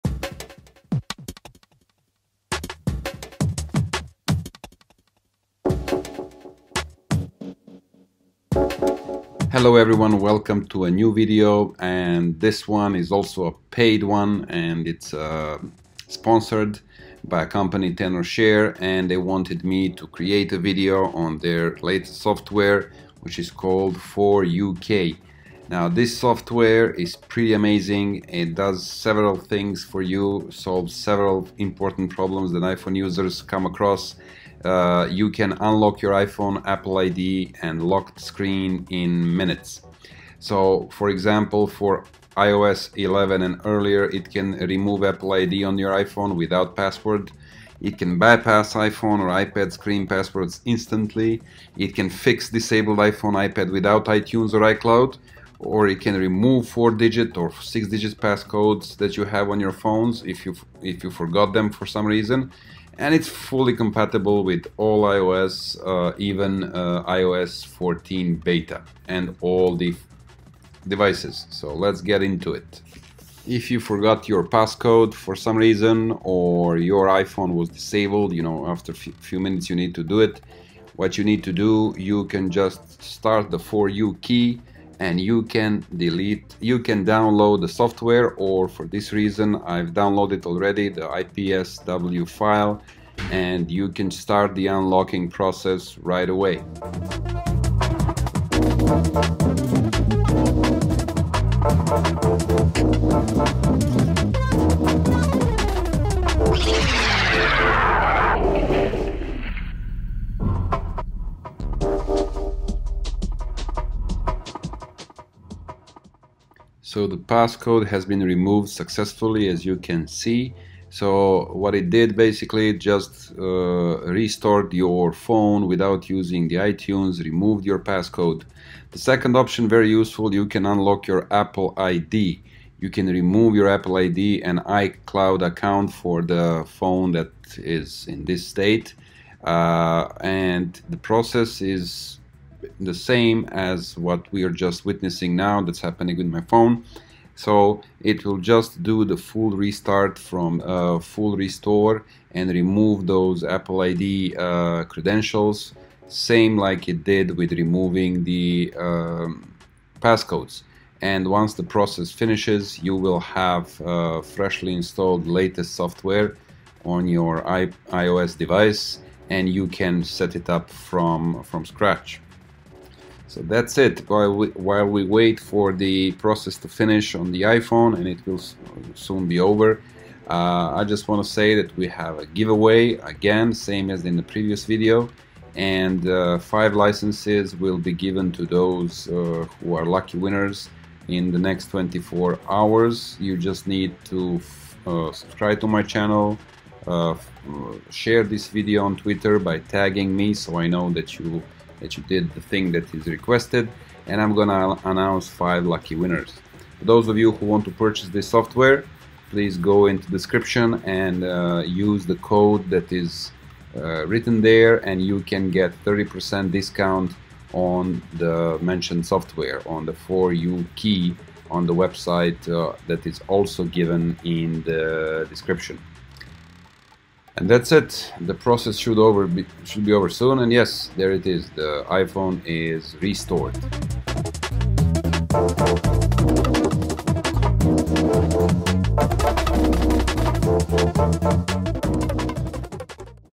Success 2020!!! / New iOS Password Unlock/Remove Apple ID
The video is about 4uKey software that helps you to remove forgotten password on your iPhone by doing a fresh restore without iTunes.